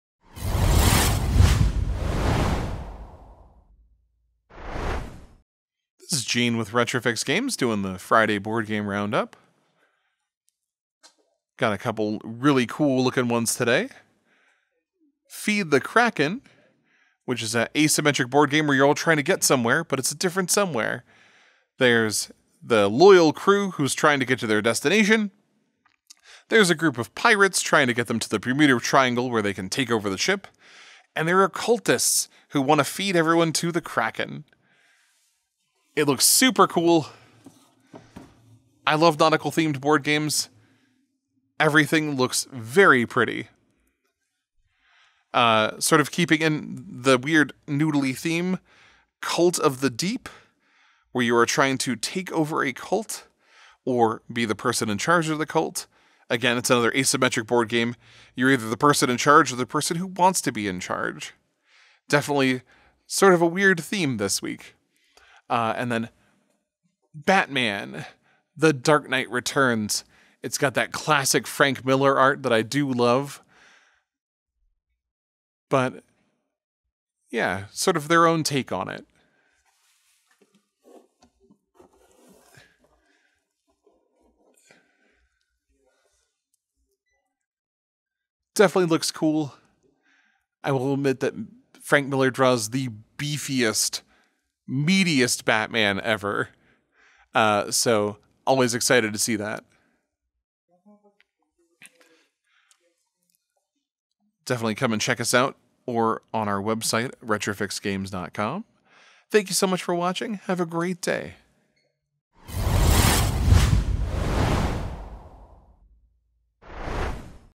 Gene's Board Game Roundup 7-15-22
Gene shows off a few of the board games that caught his eye this week.